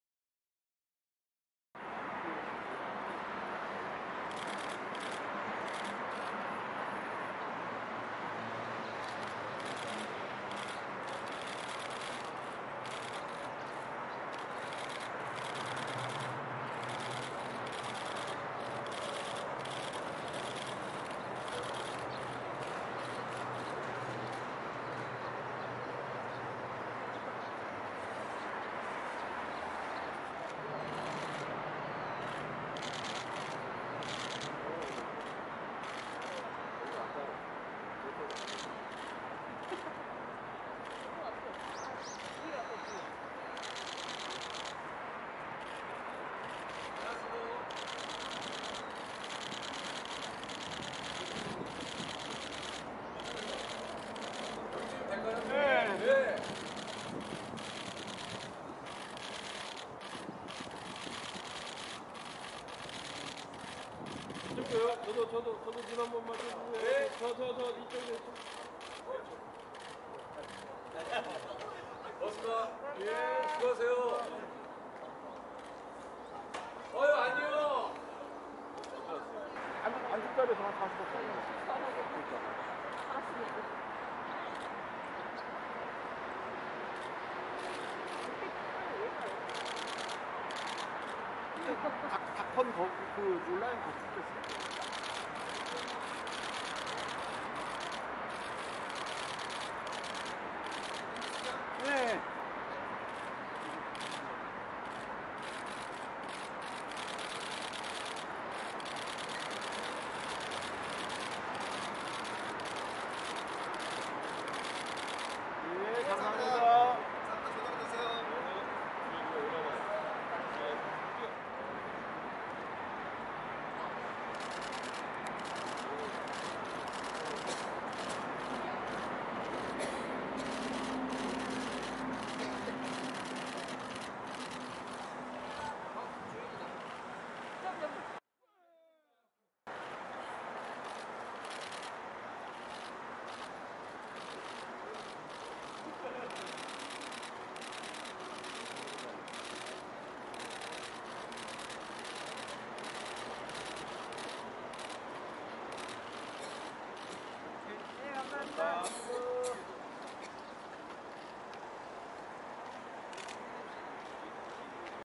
0302 KBS2 '해피투게더4' 유재석X전현무X조세호X조윤희 출근길
2일 오전 서울시 영등포구 여의도동 KBS 별관에서 KBS2 '해피투게더4' 녹화가 진행돼 전현무, 유재석, 조세호, 조윤희가 참석했다.

유재석, 전현무, 조세호, 조윤희, 해피투게더, 출근길